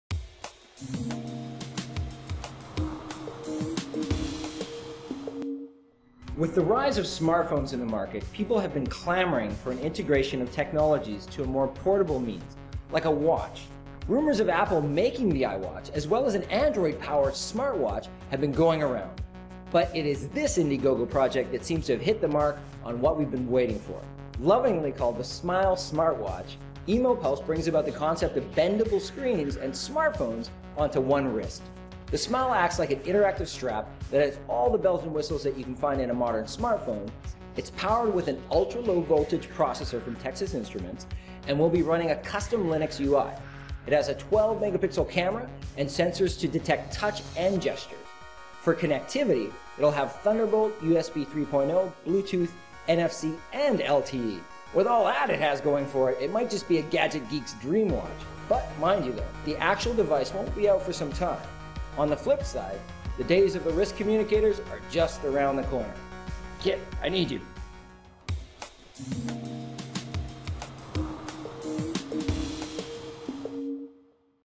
Rise of the smart watch
The Smile SmartWatch from Emopulse will feature a bendable screen and a 12-megapixel camera.